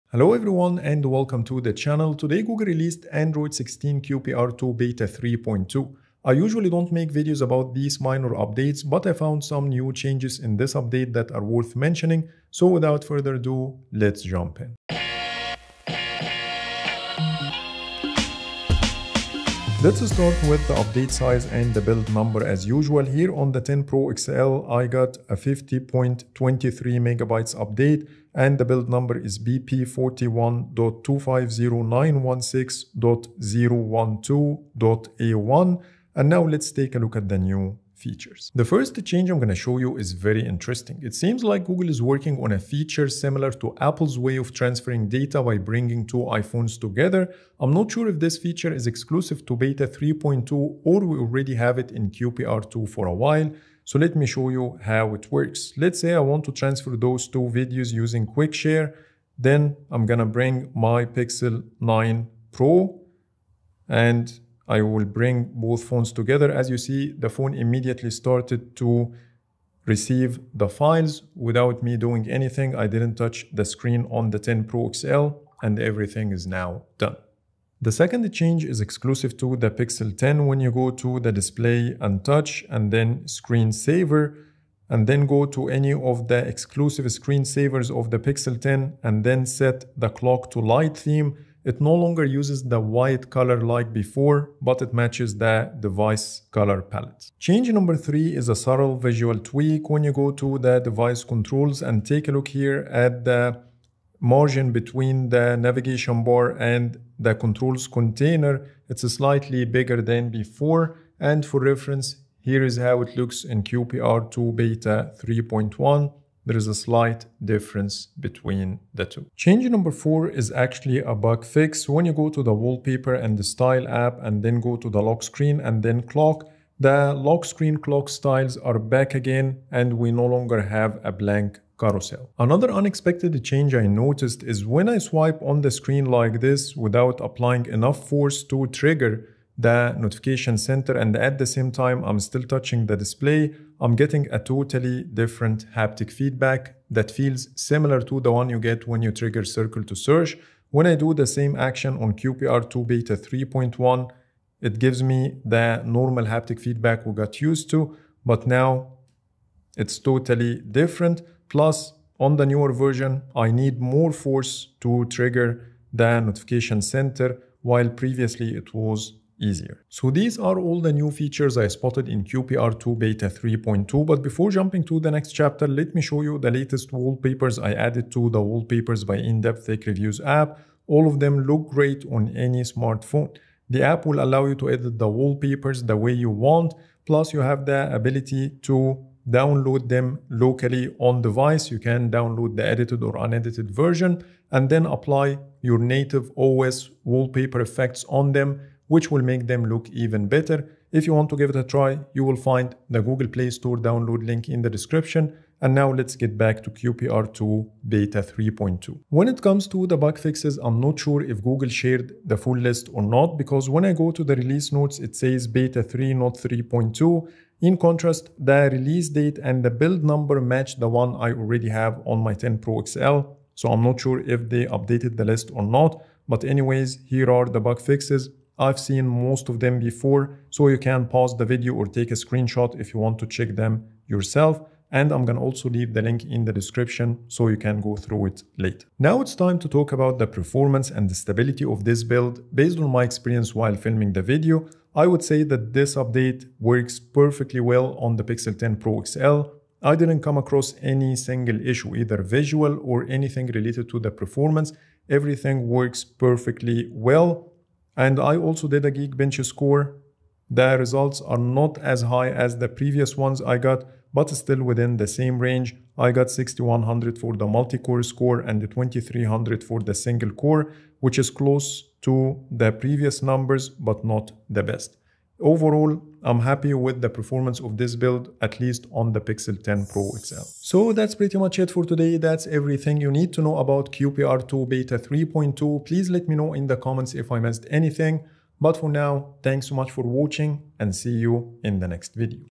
Android 16 QPR2 Beta 3.2 – Hidden Features You Need to Know!
Google has just rolled out Android 16 QPR2 Beta 3.2, and it brings several under-the-hood improvements and visual refinements to Pixel devices! 🔥

In this video, I’ll walk you through all the new hidden changes, performance optimizations, and UI tweaks that make this version smoother and more polished than ever.

This update focuses on system stability, battery efficiency, and refined animations, giving Pixel users a noticeably improved experience in daily use.

In this video, you’ll see:

The most notable feature changes in Android 16 QPR2 Beta 3.2

Performance updates

System and UI improvements

My early impressions after testing it on the Pixel 10 Pro XL

If you’re following Android’s development or love exploring what’s new on Pixel phones, this is the breakdown you don’t want to miss!

**Chapters**
0:00 - Intro
0:19 - Build & Size
0:38 - New Features
2:49 - New Wallpapers
3:29 - Bug Fixes
3:58 - Performance & Stability
4:40 - Outro